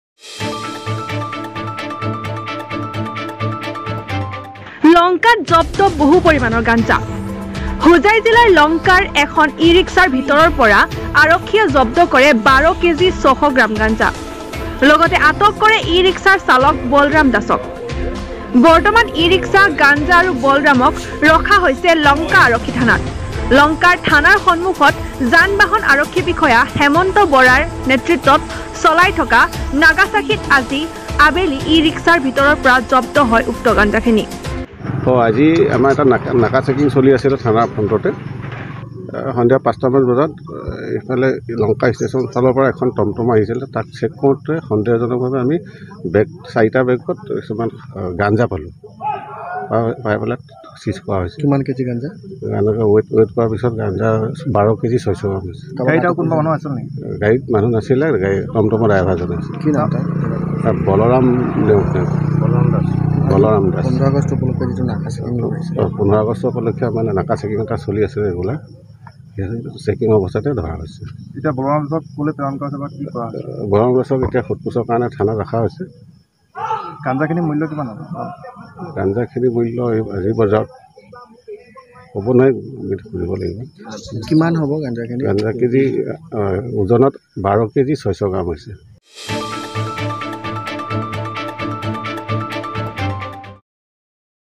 লংকাত জব্দ বহু পৰিমানৰ গাঞ্জা।
লংকাত জব্দ বহু পৰিমানৰ গাঞ্জা। হোজাই জিলাৰ লংকা চহৰত এখন ই-ৰিক্সাৰ ভিতৰৰ পৰা আৰক্ষীয়ে জব্দ কৰে ১২কেজি ৬০০ গ্ৰাম গাঞ্জা ।।

■HD Live Bureau